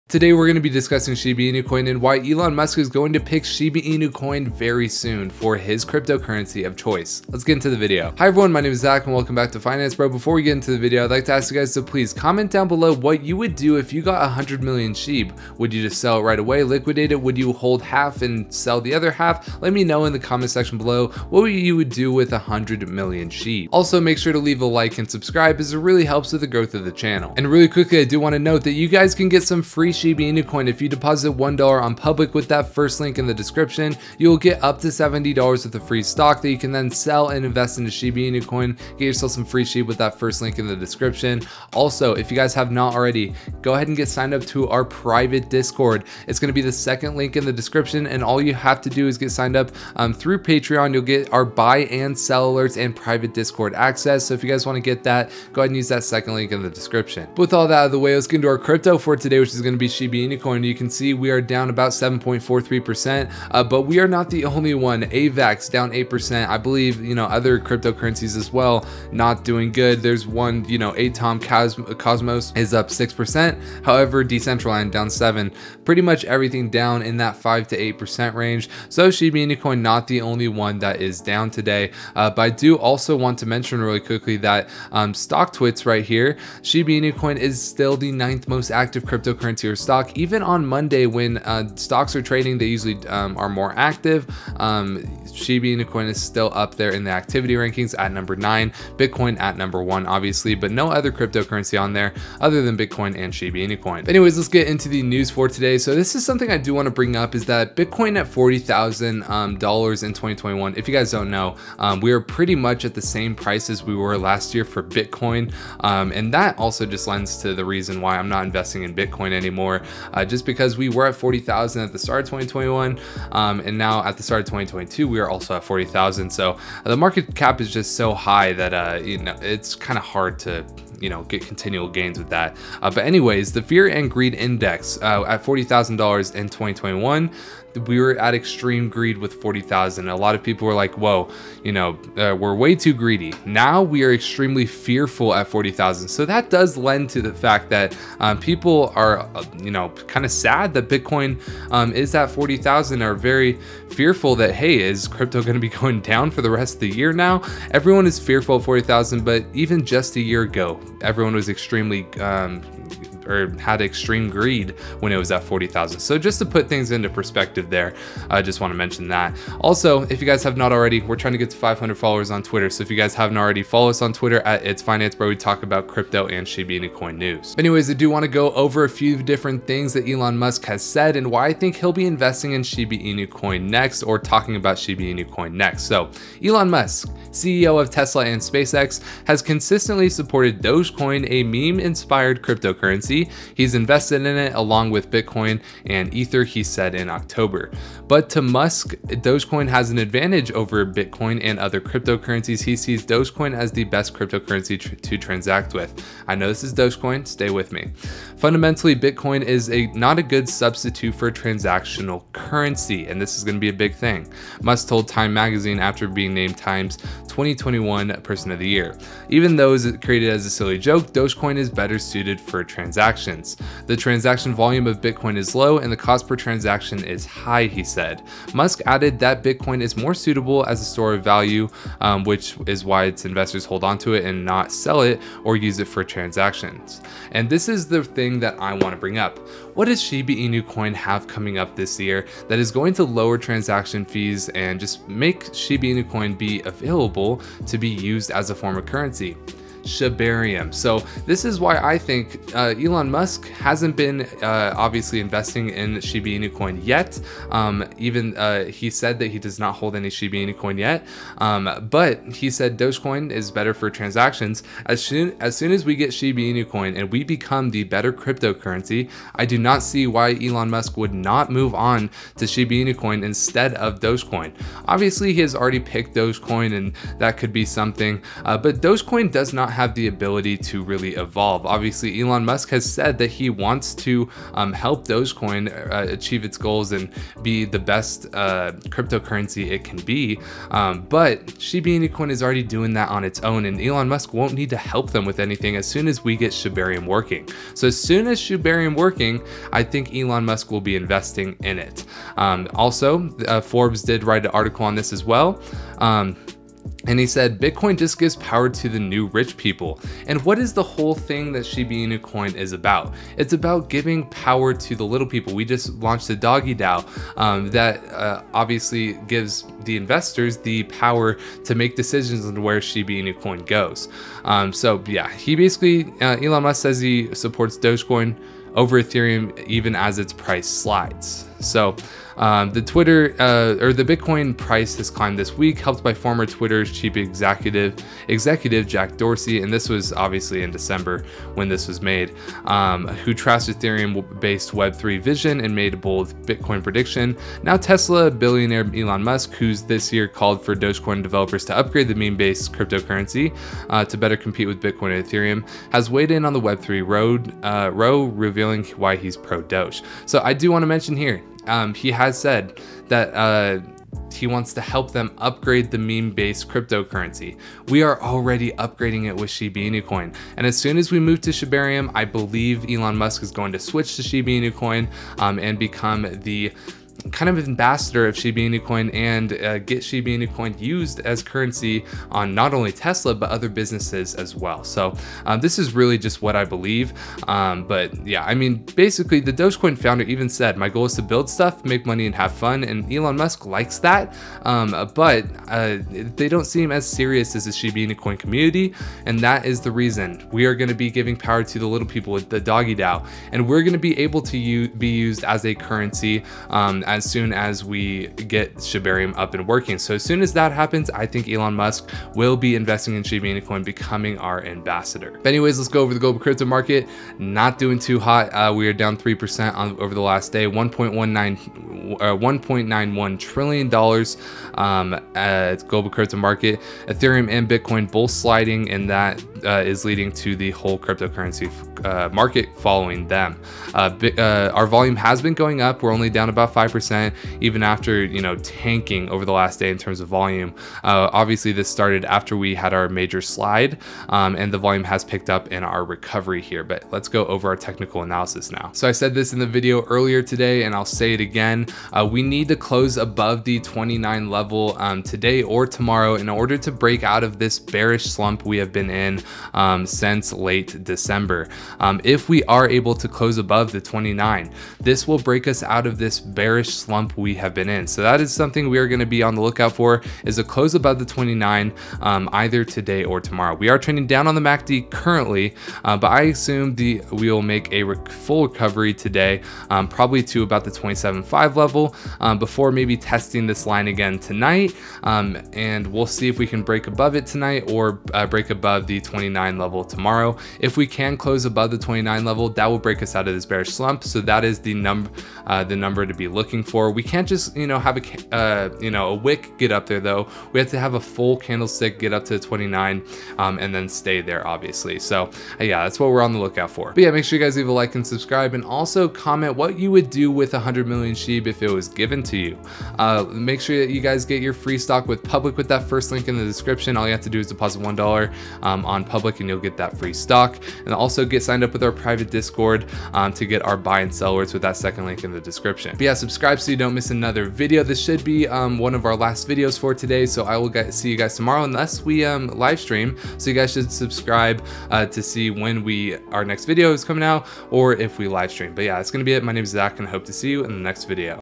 SHIBA INU COIN 🔥 ELON MUSK INVESTING IN SHIB 🚨 SHIB PRICE PREDICTION 💯
In this video, we go over Shiba Inu Coin and Elon Musk.